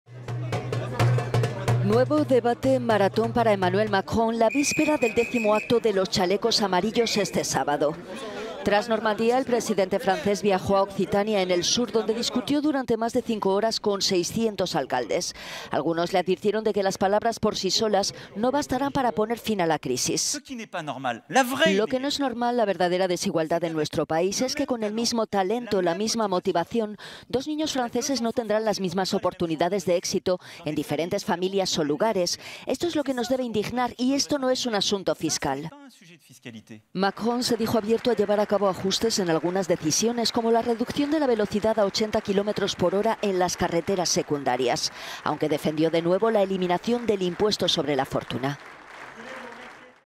Los debates de Macron no frenan a los "chalecos amarillos"
Tras Normandía, el presidente francés viajó a Occitania, en el sur, donde discutió durante más de cinco horas con 600 alcaldes. Algunos le advirtieron de que las palabras por sí solas no bastarán para poner fin a la crisis. …